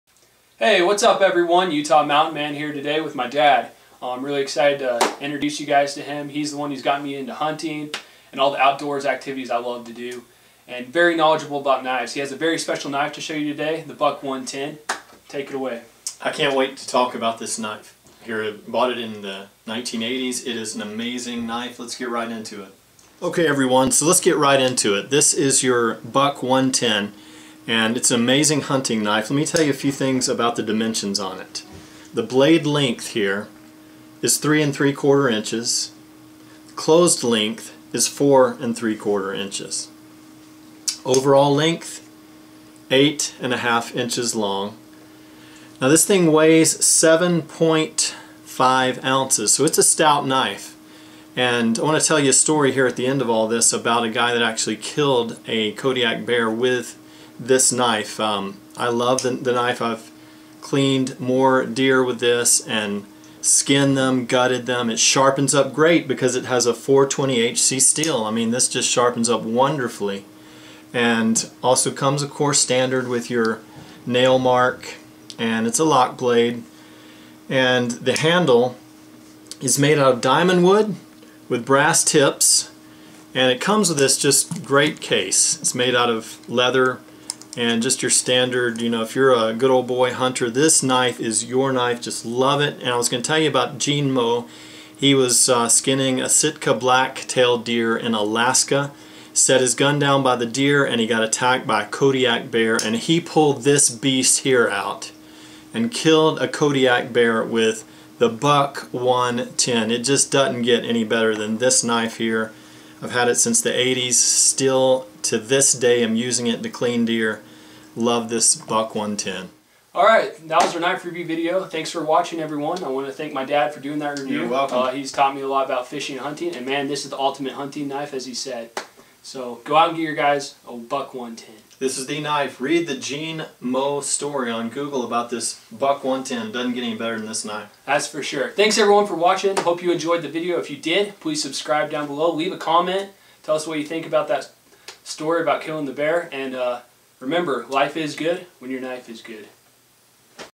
Kodiak Bear killed with the Buck 110 (Knife Review and Gene Moe Story)
The 110 Folding Hunter- A traditional favorite! Buck's Folding Hunter was created in 1963 when Al Buck decided that a revolutionary lockblade knife was needed by outdoorsmen who wanted a sturdy knife but did not want to carry a long fixed-blade knife. He was right and the result proved to be the key to the company's future. The challenge was to combine the strengths of other folding knives with locking mechanisms into a good-looking, reliable product. It wasn't easy, but after a series of refinements, the Model 110 was unveiled. Within six months, this innovation was the hottest knife in the industry. The Folding Hunter, after over 50 years, is still one of America's best-selling knives. Length 4 7/8" closed. Made in the USA.